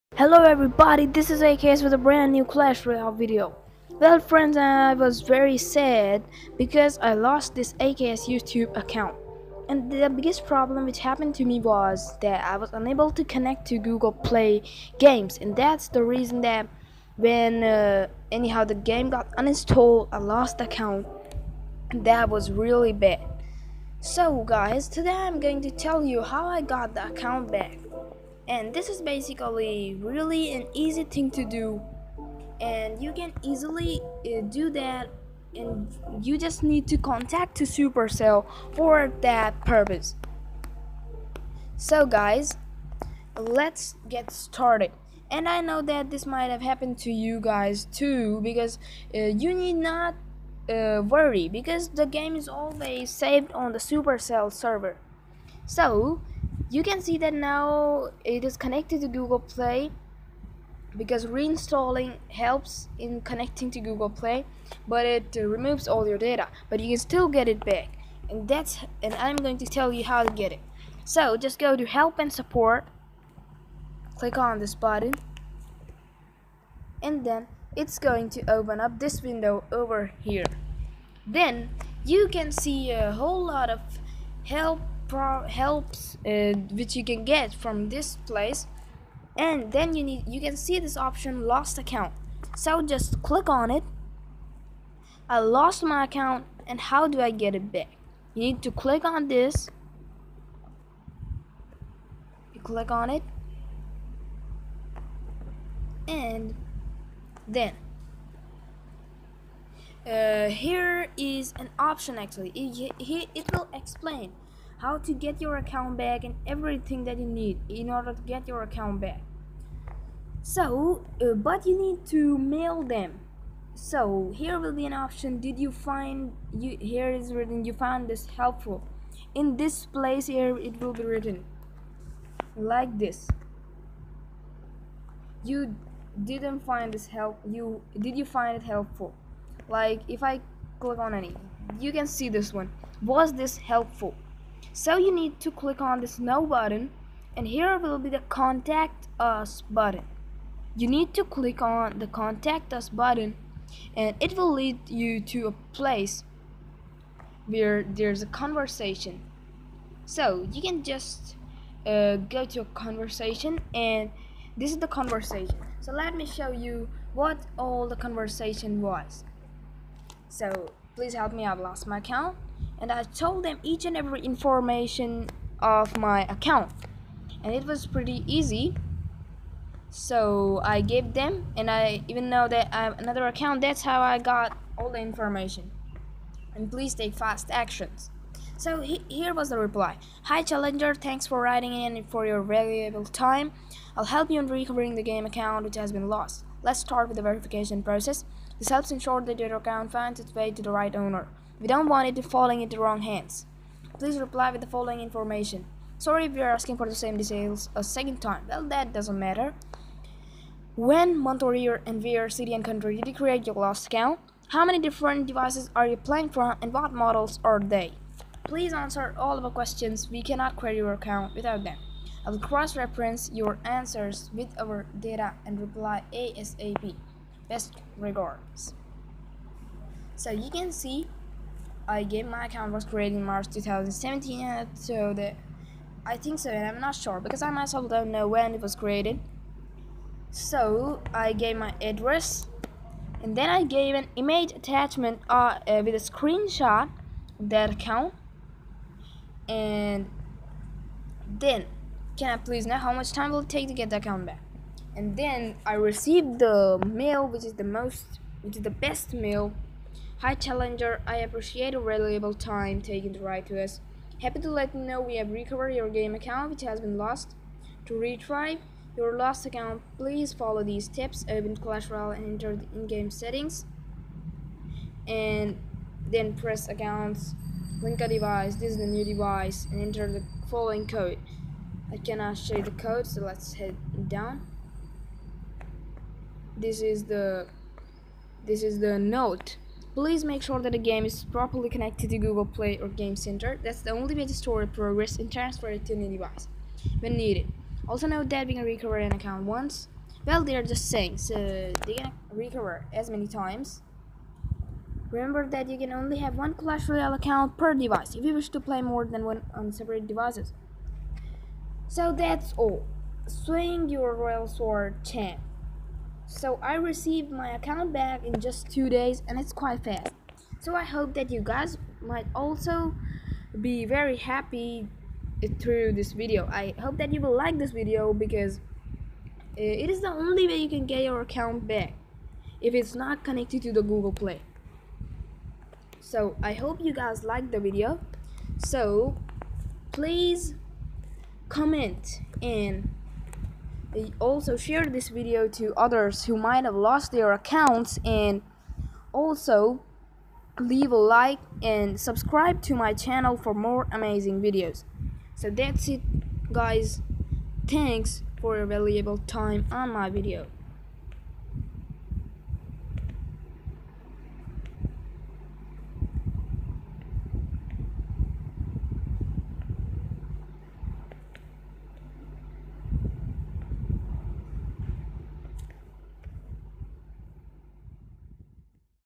How to get back/recover lost Clash Royale account. 100% working!
In this video I will tell you how you can get a lost clash royale account back easily. I got my account back in 2 days.

Thanks...